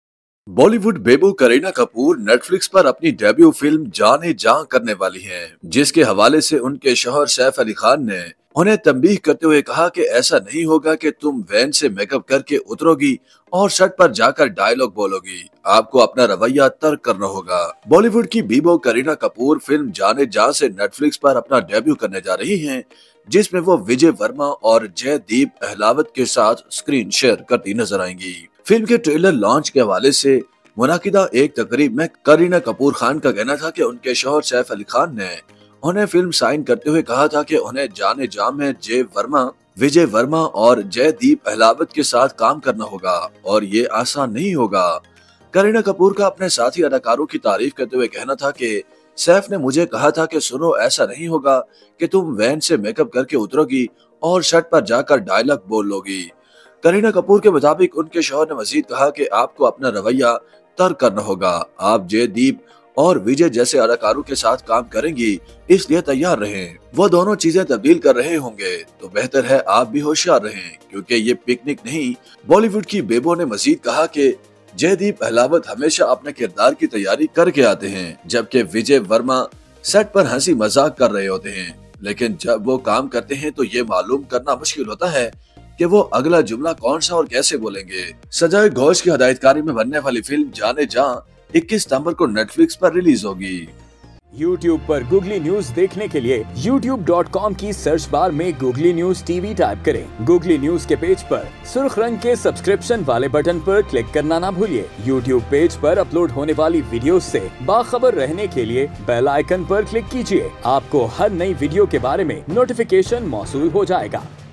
Saif Ali Khan Nai Kareena Kapoor Ko Warning Kiyun Dei? | Googly News TV
Saif Ali Khan Nai Kareena Kapoor Ko Warning Kiyun Dei?